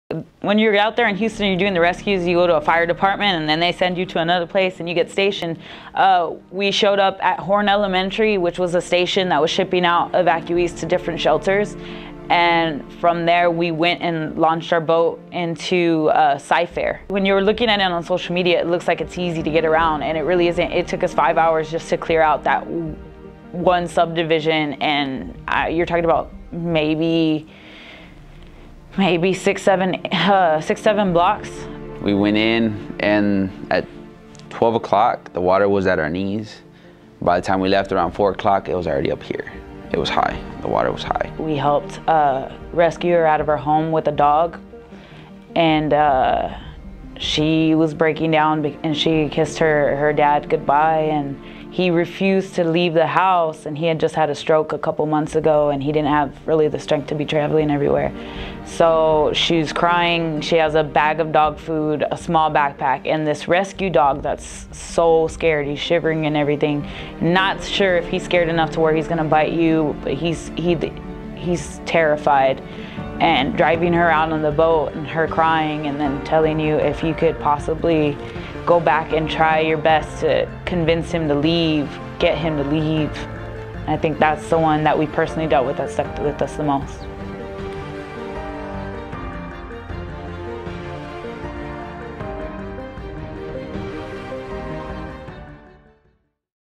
DMC Students During Hurricane Harvey
What prompts two Del Mar College students to become bass boat heroes? Hurricane Harvey, an old flat-bottom boat and Viking spirit.

Transfixed by the images coming out of Houston, Danielle Garza recounts the feeling that came over her: "As I watched what was happening in Houston I began to feel it in my heart that I had to help," she said.

Danielle's husband, Ricardo Garza, a member of the Mathis Volunteer Fire Department, said, "Honestly, I wouldn't have gone without my wife. But I told her, ‘You know what? We can do it...let's go.’"
Ricardo and Danielle hooked up their 14-foot bass boat and left Mathis for Cy-Fair, a Northwest Houston suburb, to begin evacuating people.

"When you're looking at it on social media it seems like it's easy to get around, but it's not,” Danielle said. “It took us five hours just to clear out one subdivision."

Danielle is the incoming Student Government Association president as well as a biology major. Ricardo frequently volunteers at Del Mar College and is a business administration major.